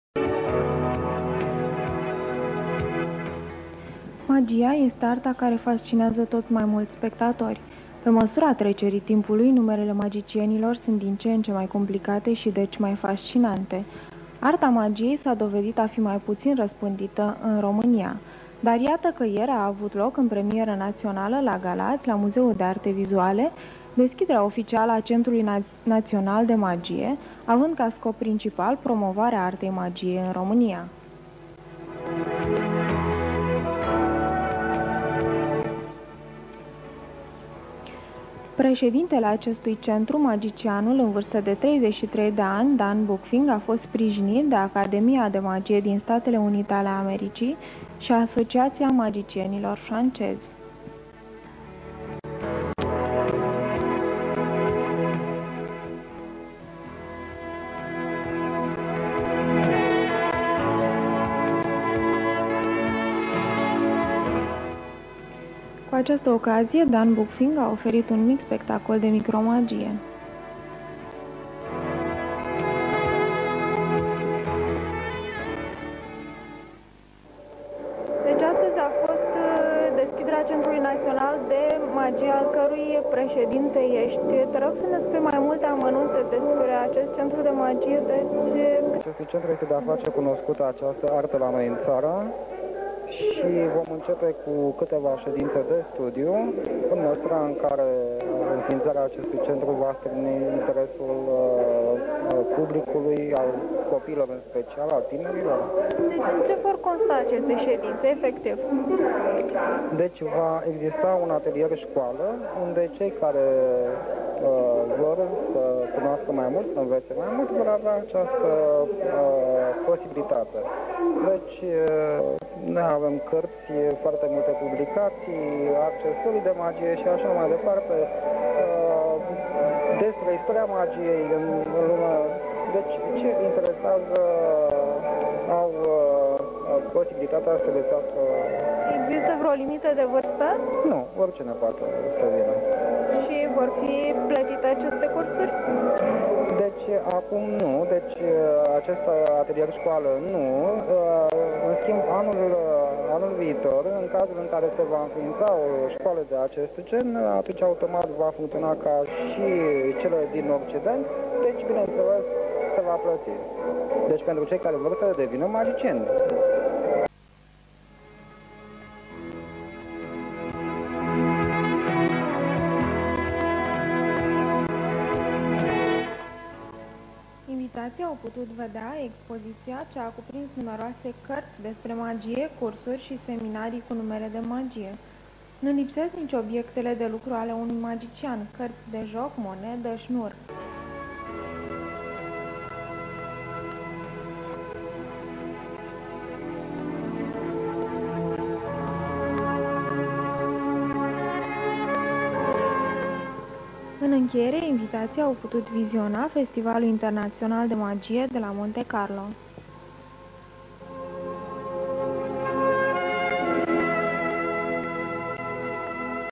ILUZIONIST-SHOP Arhiva Video 1995 CENTRUL DE MAGIE GALATI.MP4
Ideea formarii unei asociatii a artistilor iluzionisti/magicieni apartine artistului Dan Bucfing,care in 1995 infiinteaza pentru prima data in Romania Centrul National de Magie.Intre membrii s-au aflat la acea data Ionut Stoian care mai tarziu avea sa fie cunoscut publicului sub numele de Johannes si  celebrul prestidigitator Sasu Benedict.

Centrul National de Magie a functionat in perioada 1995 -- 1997 cand iluzionistul  Dan Bucfing a intreprins  timp de 10 ani turnee internationale,pe mai multe continente